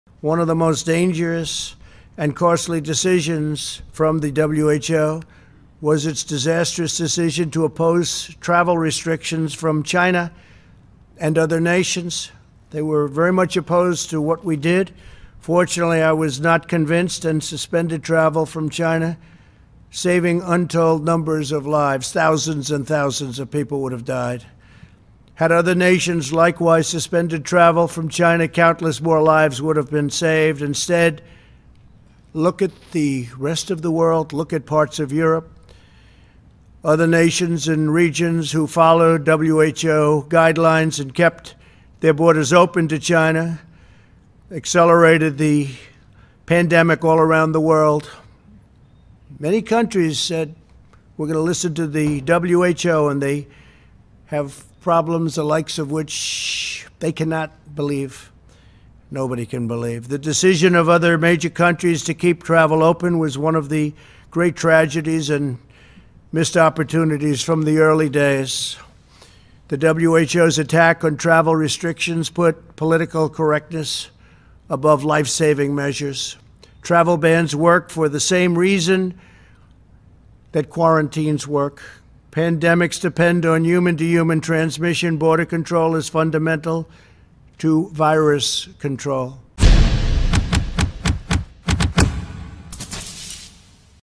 Trump: WHO’s Attack on Travel Bans Put Political Correctness Above Saving Lives
President Trump said Tuesday the World Health Organization’s (WHO) attacks on imposing travel bans during the coronavirus crisis “put political correctness above life-saving measures.”